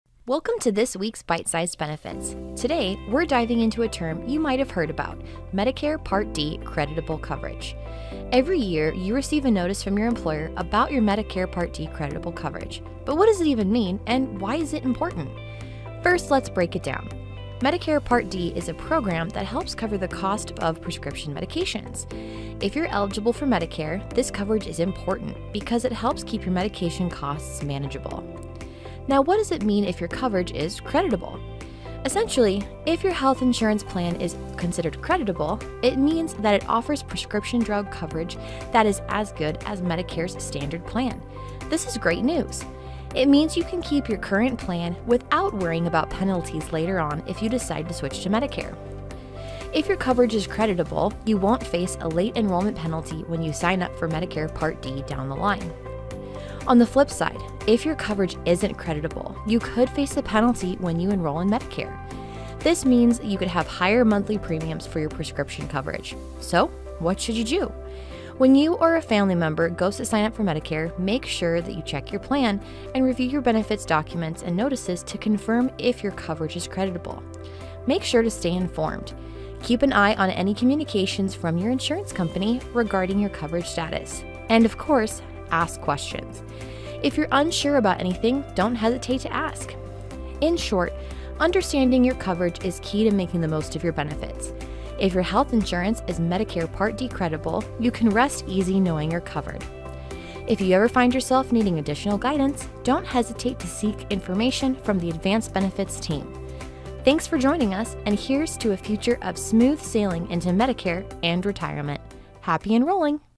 What is Medicare Part D Creditable Coverage? BITE SIZED BENEFITS
Improving Business. Improving Lives.™

Every year you receive a notice from your employer about your coverage and whether or not it is Medicare Part D Creditable. But what does it mean to have creditable or non-creditable coverage? This video explains what it means when your plan is creditable under Medicare and what else to consider.